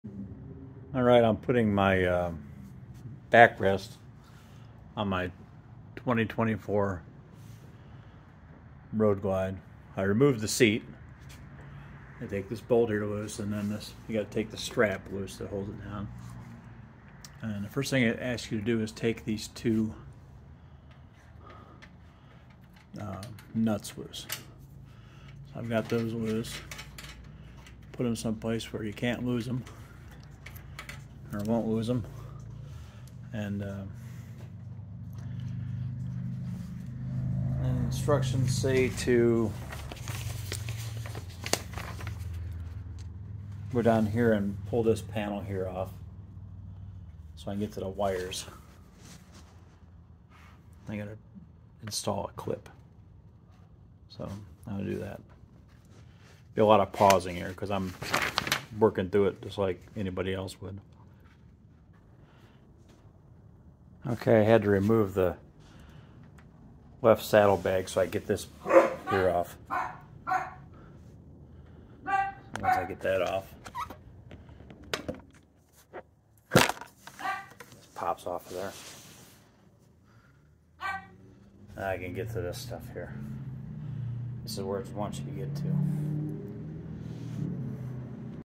2024 Roadglide adjustable backrest install.
installing of the adjustable backrest on a 2024 Road glide.